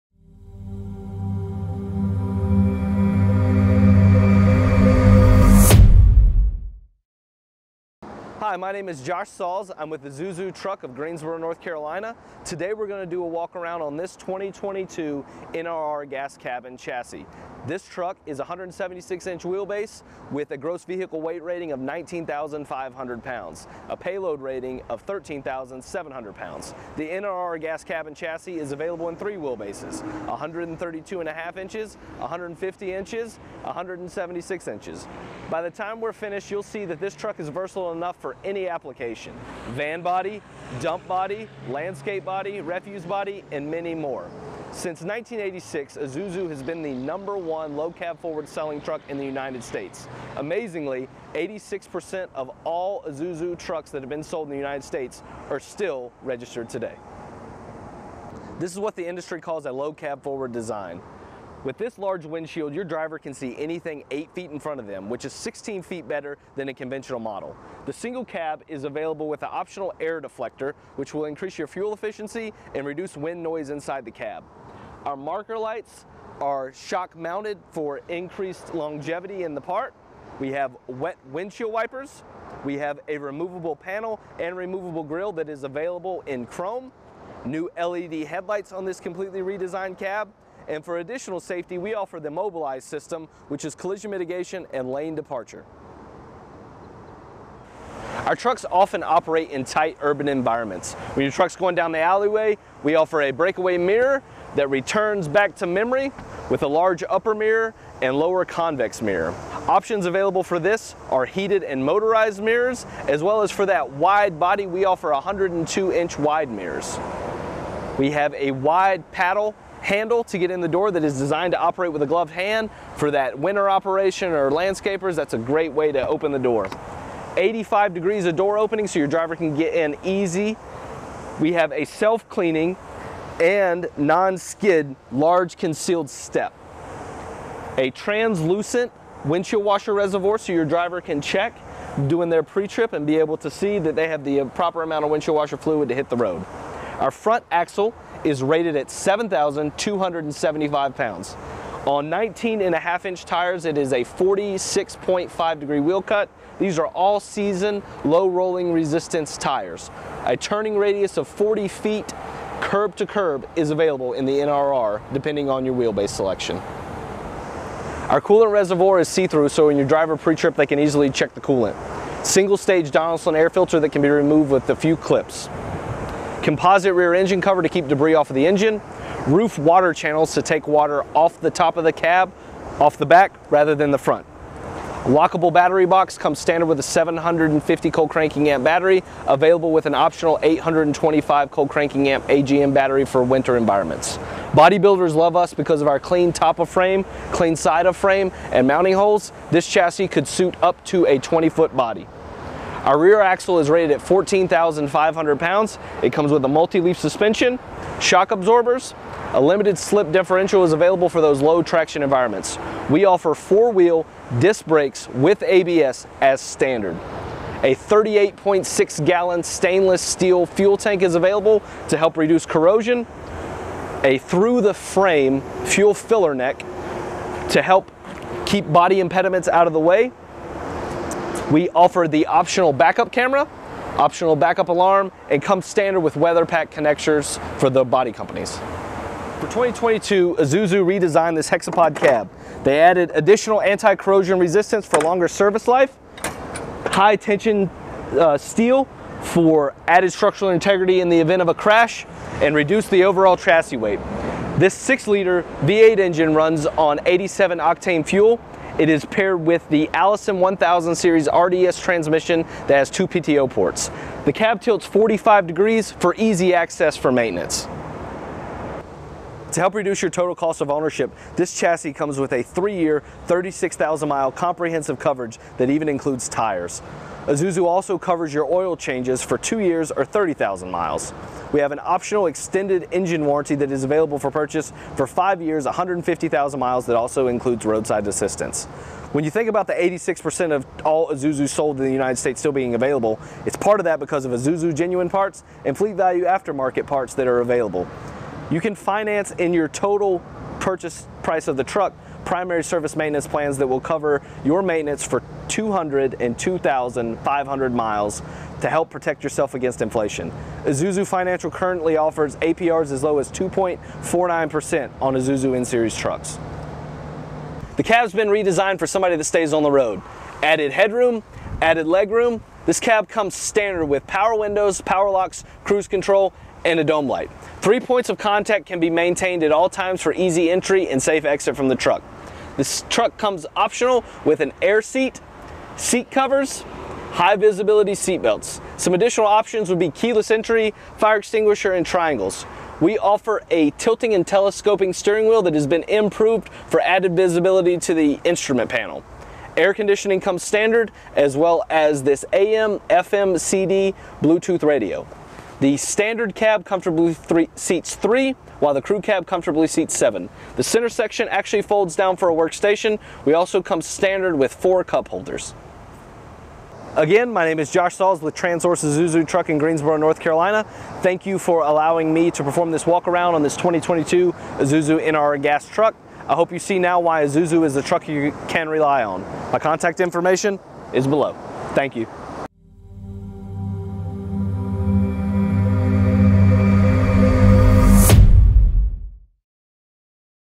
TranSource Isuzu NRR Walkaround Video with Josh Sauls
Josh Sauls reviews the all new 2022 Isuzu NRR Gas cab and chassis for the 2022 Isuzu walkaround competition